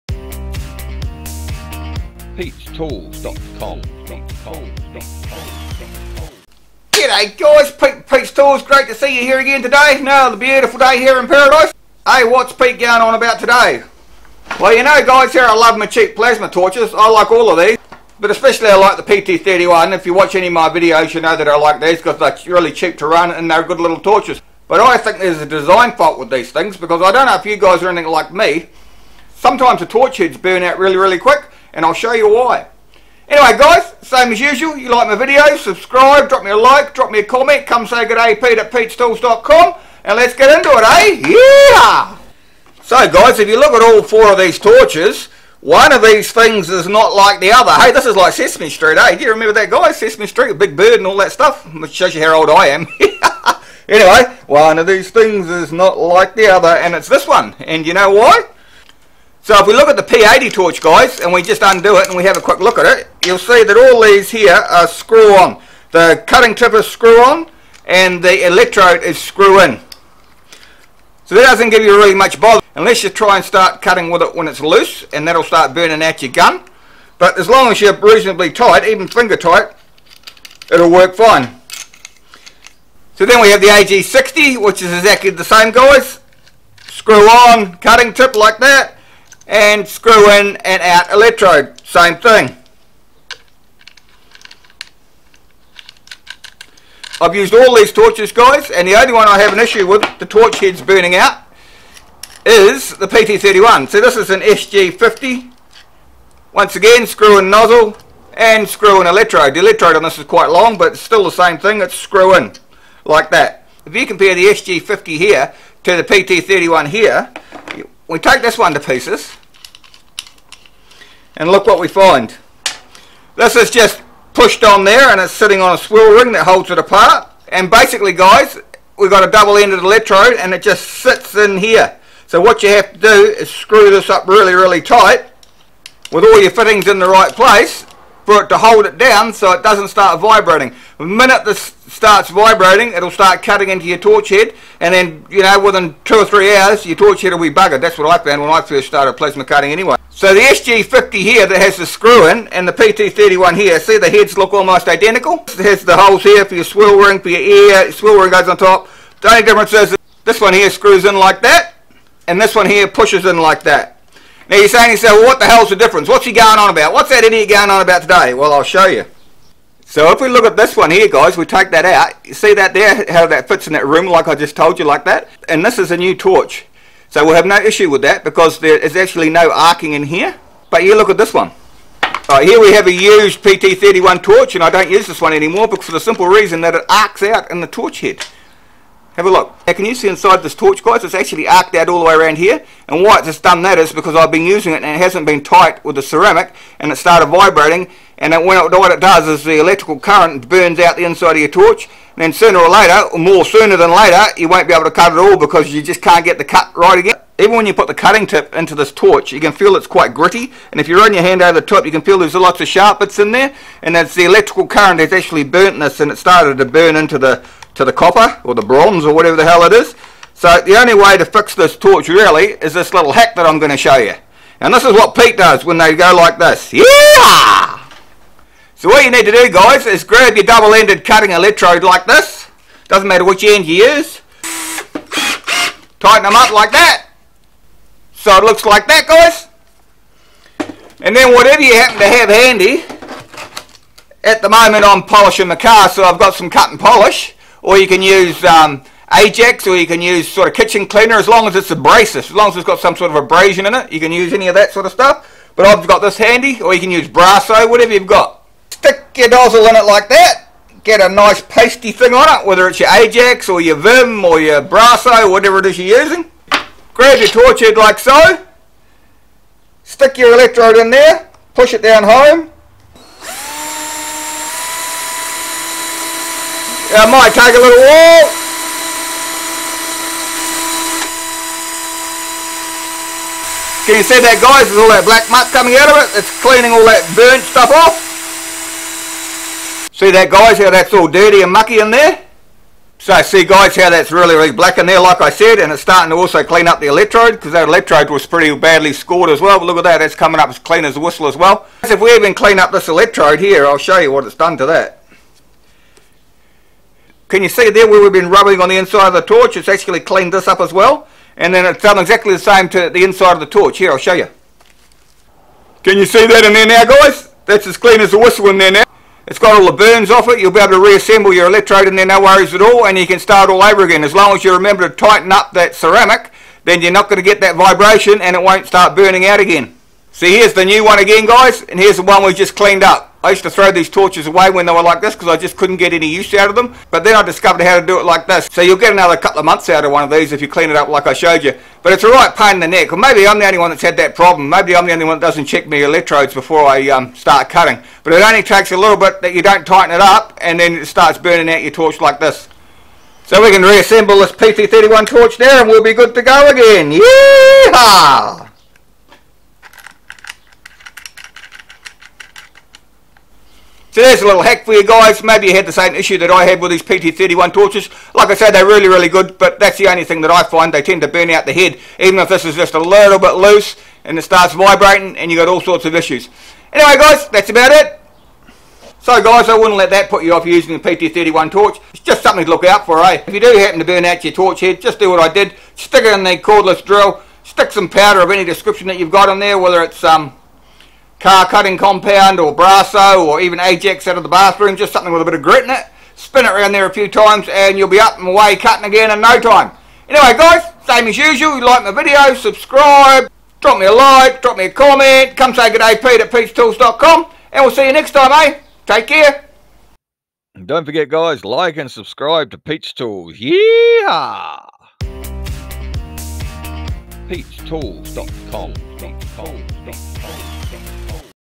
Plasma Torch Repair (No Money Needed)
Plasma torch repair This is my  pt 31 lg 40 plasma torch hack if you are running a cheap plasma torch you might find you have trouble with your plasma cutter torch head assembly burning out I have only found this issue with the pt-31 or lg-40 torch. It is because this type the cutting electrode only pushes into the torch head .Ware as most other styles the plasma cutter consumables screw into place therefor not being able to work loose while cutting your steel. If your consumables work loose then you run the risk of burning out your plasma torch in no time what so ever. I burnt out many torches before I realized what was happening use my simple plasma torch hack to repair your torch at no cost and be up and running again in no time.